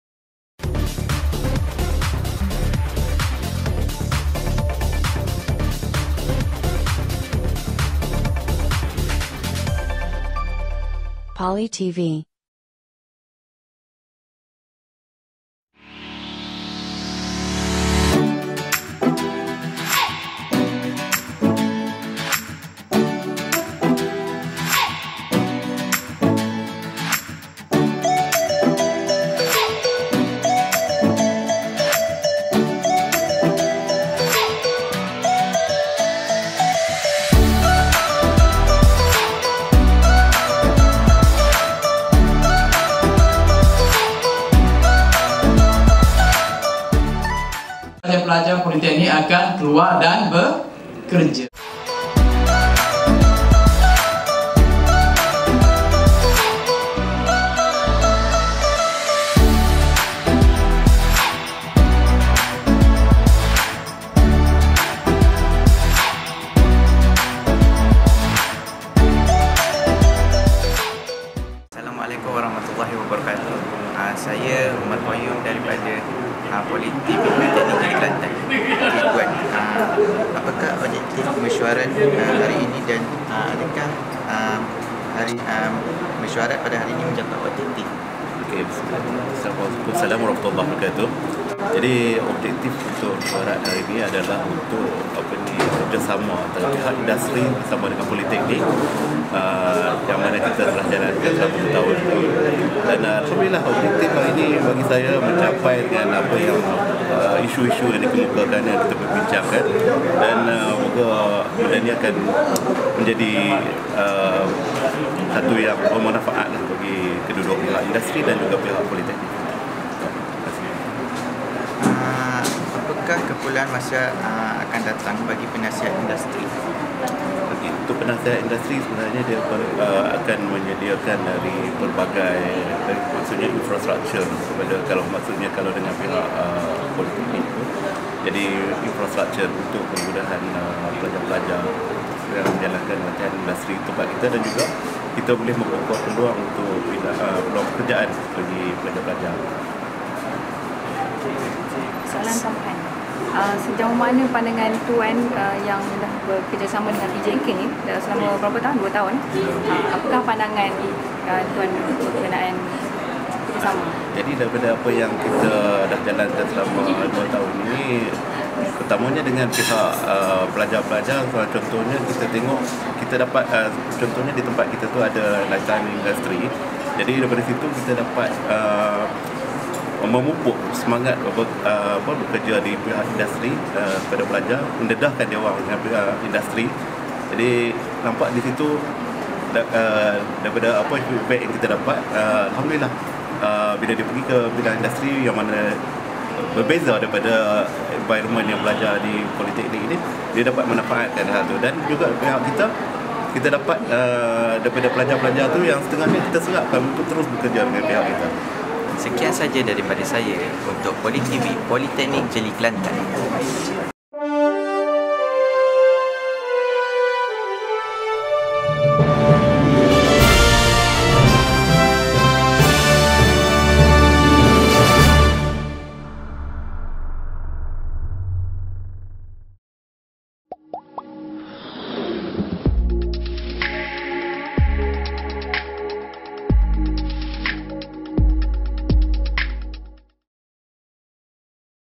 PoliTV PJK 2019 : Mesyuarat JK Penasihat Industri & Karnival Kerjaya
Pada 24 September 2019 telah berlangsung satu Mesyuarat Perkongsian Kepakaran Jawatankuasa Penasihat Industri bagi Diploma Agroteknologi Politeknik Jeli Kelantan, serta diadakan juga Karnival Kerjaya pada hari yang sama.